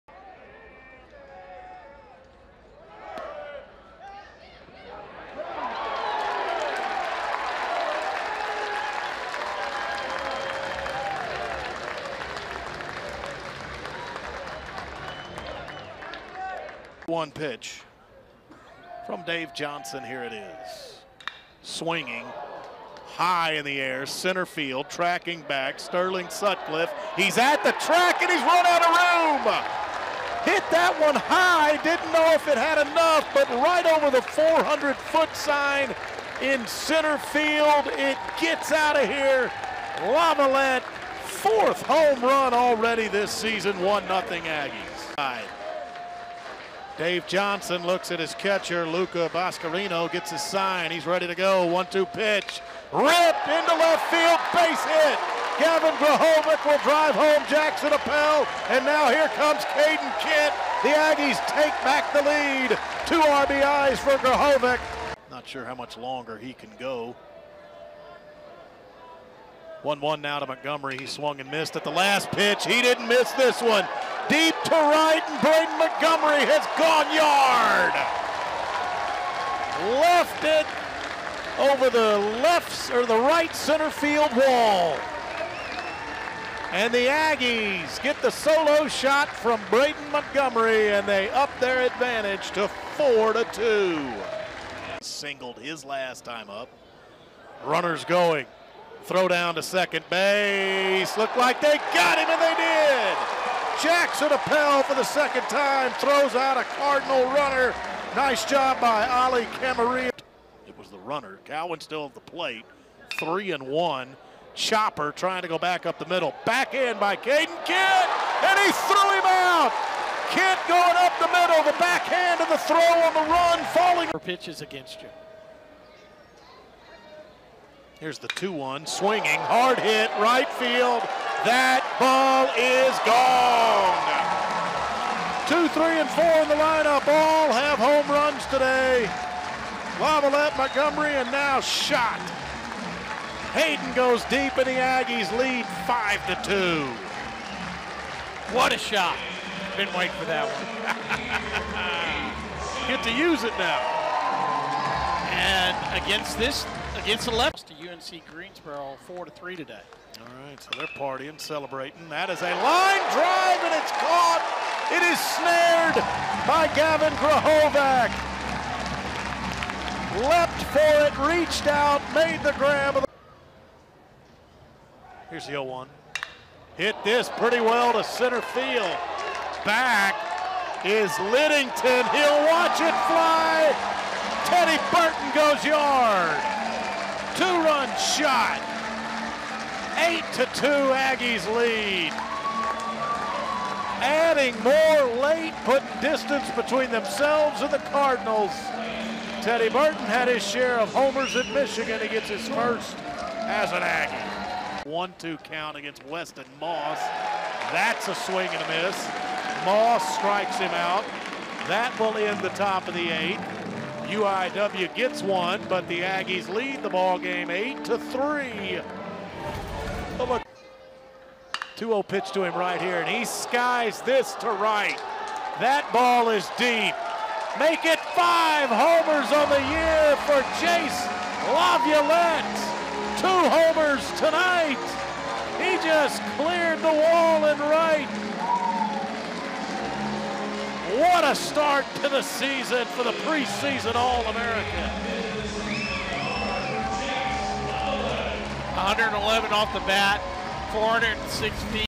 Highlights: Texas A&M 9, UIW 3
Watch Aggie baseball's highlight package from the 9-3 midweek win over Incarnate Word.